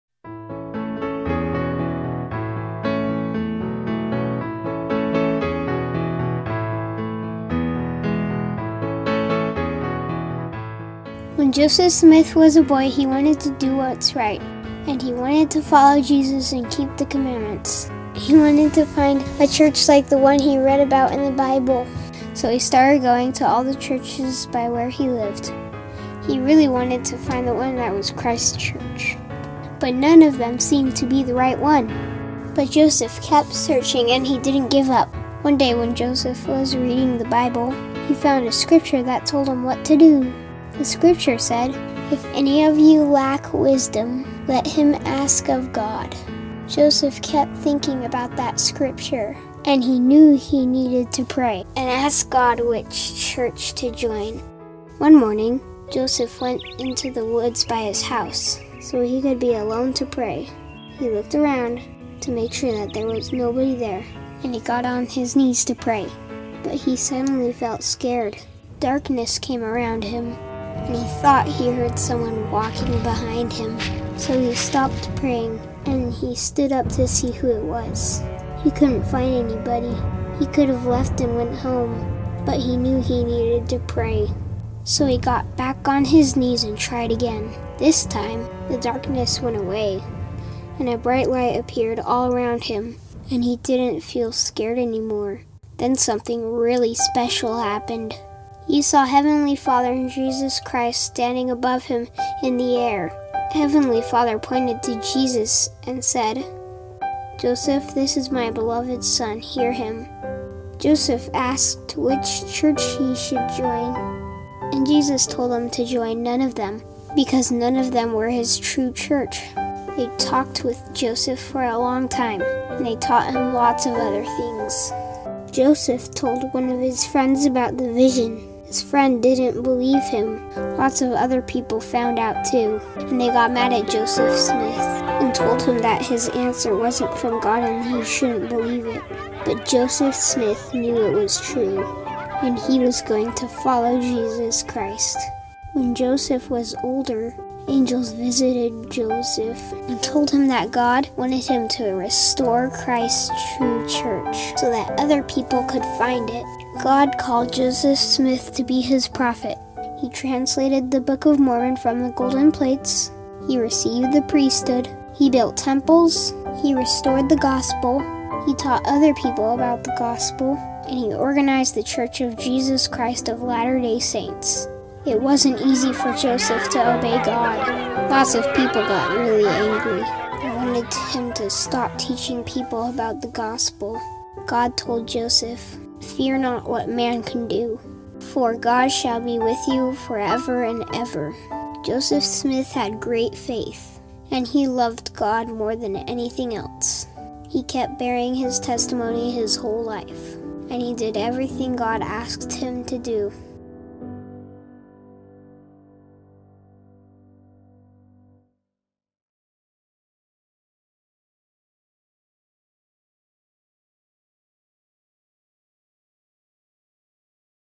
Joseph Smith and the First Vision | Family Home Evening Lesson
This animated video summarizes the events of Joseph Smith's first vision in a simple manner so that children can follow along and understand. Enjoy this video as part of your Family Home Evening lesson. Also perfect for Sunday School or Primary!